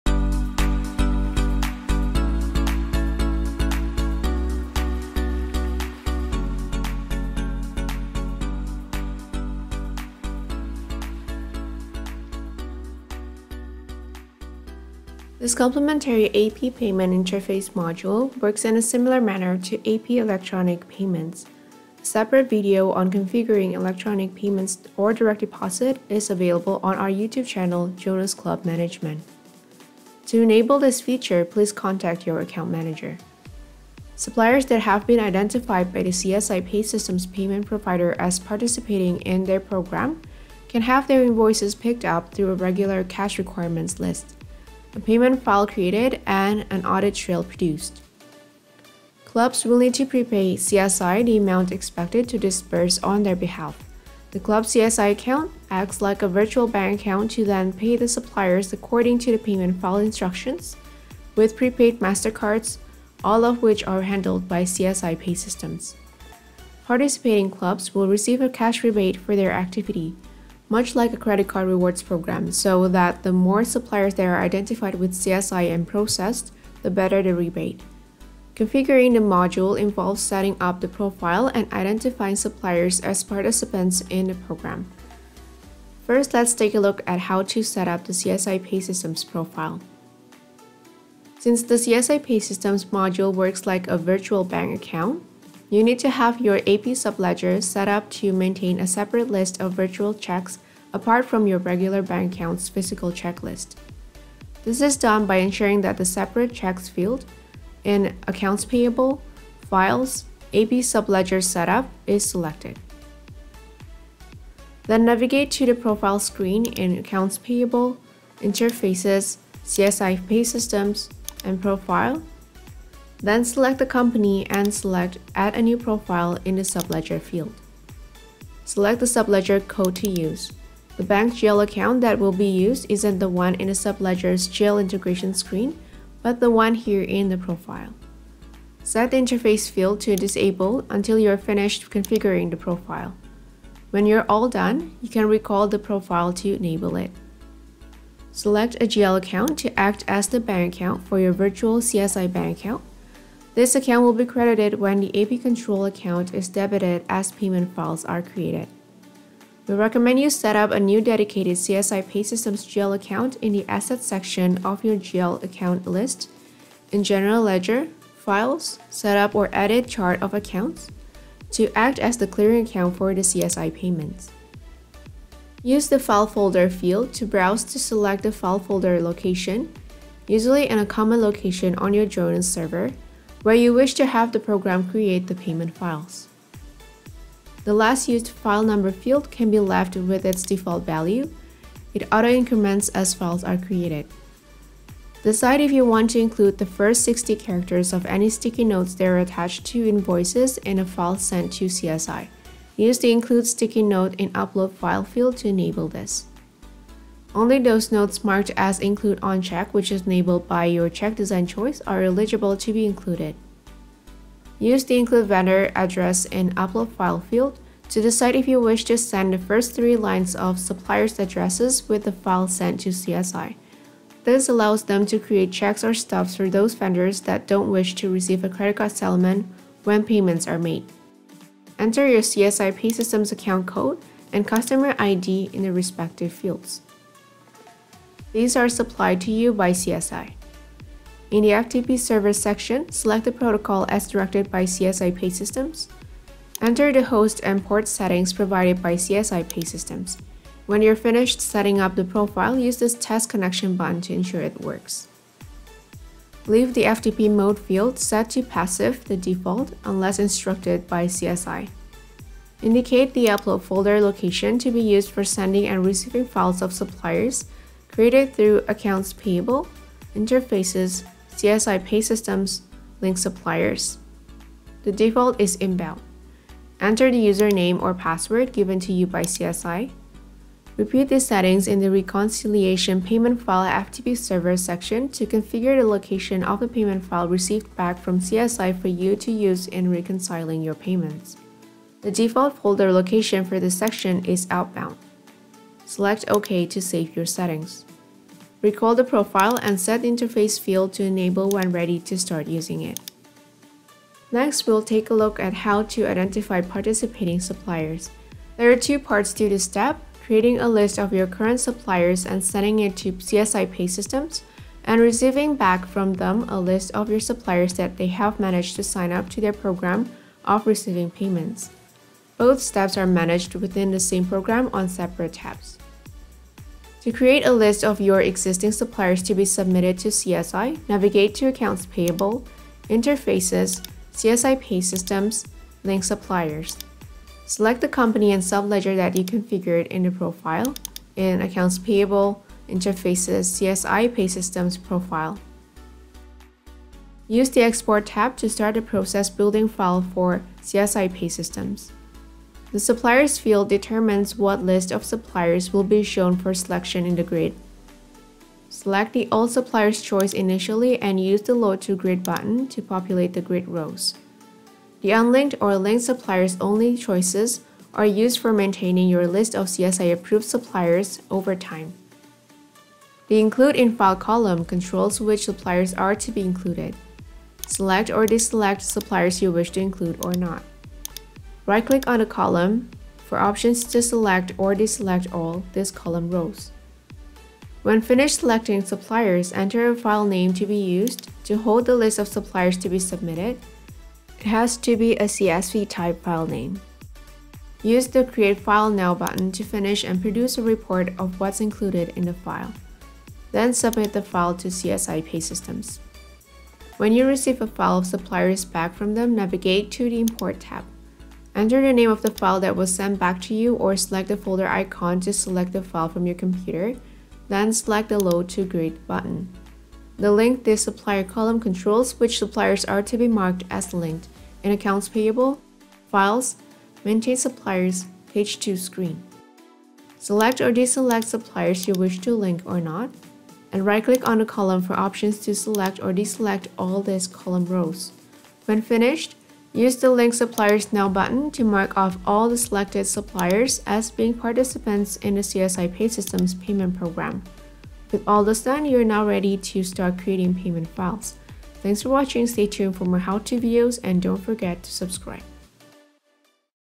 CSI Paysystems - Profile Setup and Identifying Participating Suppliers
This complimentary A/P payment interface module works in a similar manner to A/P Electronic Payments. To enable this feature, please contact your account manager.

Suppliers that have been identified by the CSI Paysystems payment provider as participating in their program can have their invoices picked up through a regu­lar Cash Requirements List, a payment file created and an audit trail produced.

Clubs will need to pre-pay CSI the amount expected to disburse on their behalf. The club’s CSI account acts like a virtual bank account to then pay the suppliers according to the payment file’s instructions with prepaid MasterCards (all of which are handled by CSI Paysystems).

Participating clubs will receive a cash rebate for their activity (much like a credit card rewards program, so that the more suppliers that are identified with CSI and processed, the better the rebate).

Configuring the module involves setting up the Profile and identifying suppliers as participants in the program.

Content:
0:00 Introduction
1:30 A/P Subledger Setup
1:54 Profile Setup
2:10 Jonas Settings
3:51 CSI Paysystems Settings
5:04 Identifying Participating Suppliers
5:26 Creating a list of your current suppliers
6:47 Importing the File from received back from CSI Paysystems
7:00 Link this supplier